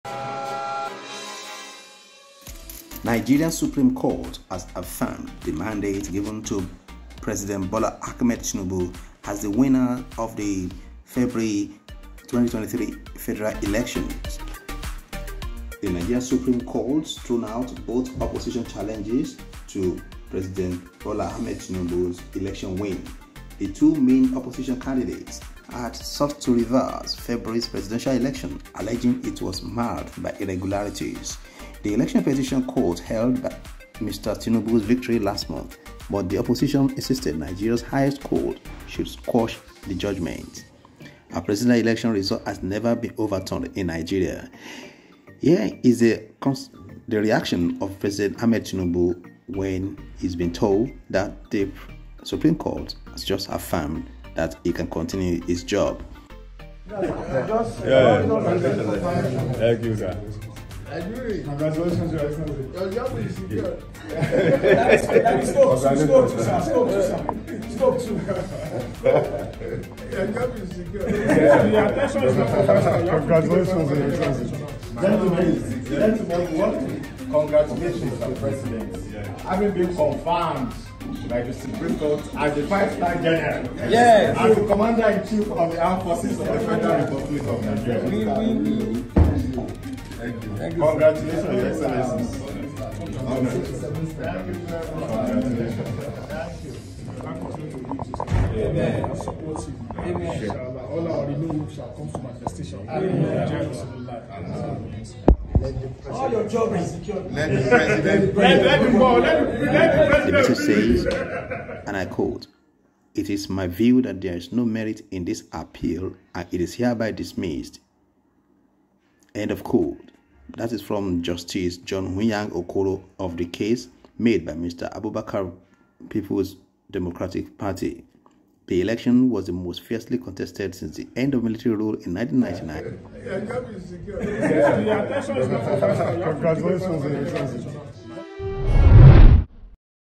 TINUBU'S REACTIONS INSIDE ASO ROCK OFFICE AS SUPREME COURT DISMISSED ATIKU / OBI APPEALS
Nigerian Supreme Court affirms President Tinubu's election win